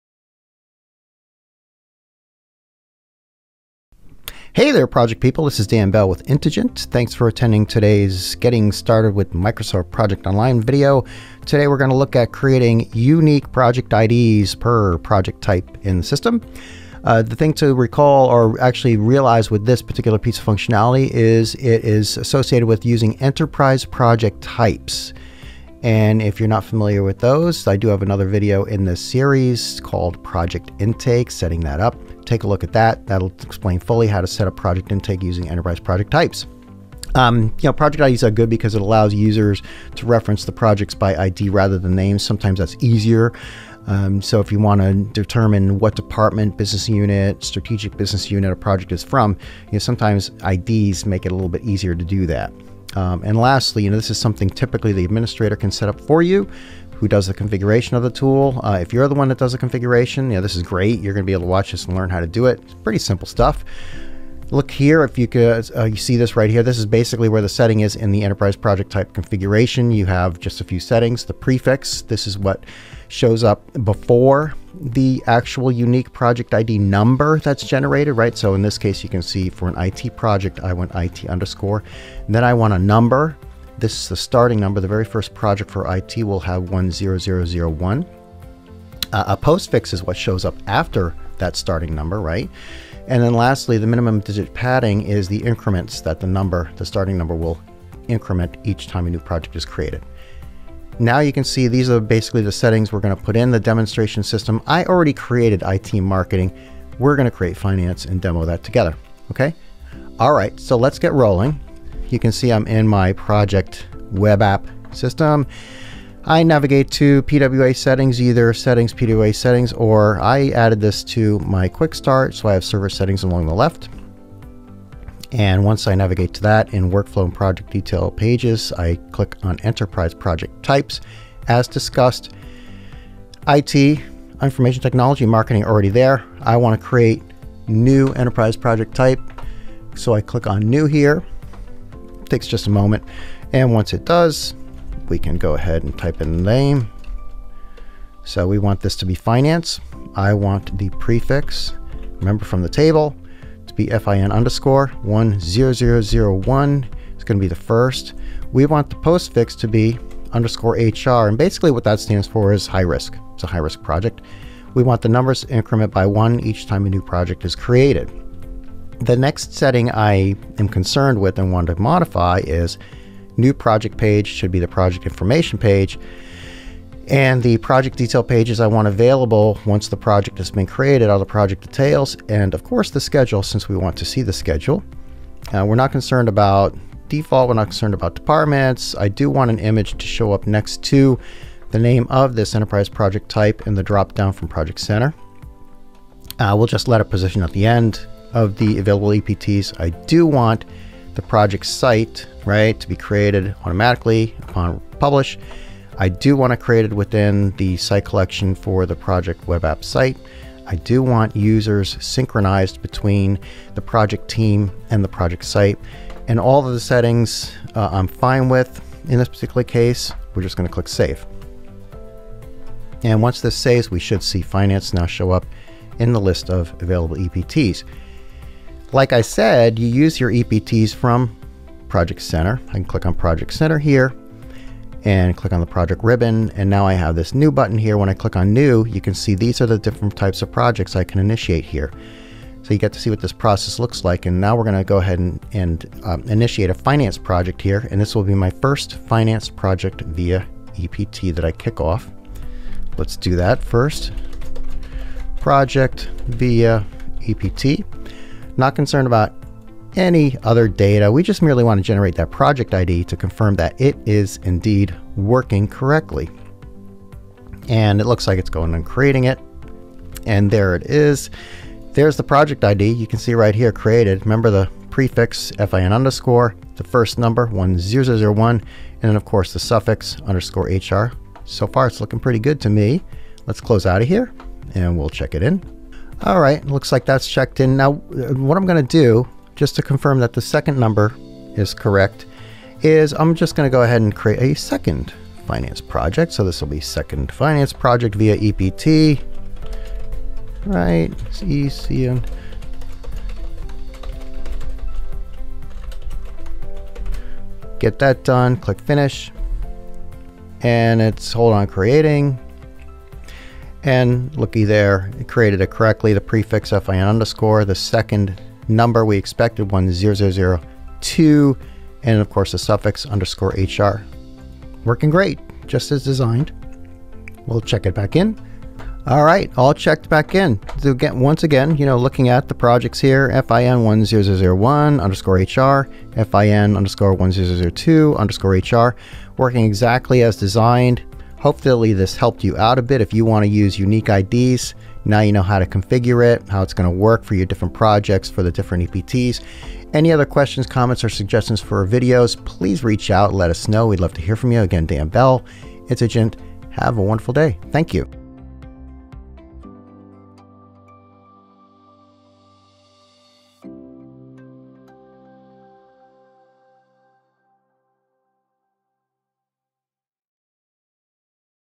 Getting Started with Microsoft Project Online (05 - Create a Unique Project ID for Projects)
Create unique Project IDs for  projects in Microsoft Project Online
In this video we demonstrate setting up unique project ids for 3 different project types in MS Project Online.

I person with the appropriate permissions to configure MS Project Online can configure Microsoft PPM to generate a unique Project ID for each new project request coming in via an Enterprise Project Type (EPT).  It is within the configuration settings of an EPT that the unique Project ID may be set up.  As an organization, you define the properties you would like used to generate Project IDs across the company.

A unique Project ID can help Project Managers, Executives, and other stakeholders more easily identify projects across the organization in Office 365 Project Online.

How to Specify Project ID settings when configuring an Enterprise Project Type in Project Online
Withing Project Web App, select Settings - PWA Settings, or Server Settings in the left navigation pane.

On the Server Settings page, in the Workflow and Project Detail Pages section, click Enterprise Project Types.

Either edit an existing Enterprise Project Type by clicking on it's name or click the New Enterprise Project Type button.

(If editing an Enterprise Project Type, go to item x below)

On the New Enterprise Project Type page, specify a Name for your new Enterprise Project Type.  Try to use a name that is descriptive for the type of project being created.

In the Description field, enter something that will help the user determine if this is the correct type of project to use for their purposes.

In the Project ID section, enter information that will be used to create a unique Project ID for the projects that are created via this EPT:

In the Prefix field, enter characters start each Project ID. For example, IT_ might be used for Information technology projects.  This field is optional.

In the Starting Number field, enter a number that will be used as the first number for Project IDs that will be generated for this EPT. Enter 10001 if you want the first Project ID to be 10001. This field is required.

In the Postfix field, enter characters that will show up after your Project IDs that are created for this EPT. For example, if this EPT is used to create only low-risk projects for the Finance department, you could enter _LR. This field is optional.

In the Minimum Digit Padding field specifies the padding prior to the first number you specify.  For instance, if you specify a starting number of 1 and padding of 5, the first 5 numbers will be 000001, 000002, 000003, 000004, 000005.  In my opinion I would prefer to use a starting number of 100001 with a padding of 1 which seems to me to keep the number of digits consistent for a longer time.

When creating new projects in Microsoft Project Online, it may be beneficial to have unique Project IDs generated for different types of projects.  This could potentially make it easier for users to identify projects by department, business unit, or some other mechanism within an organization.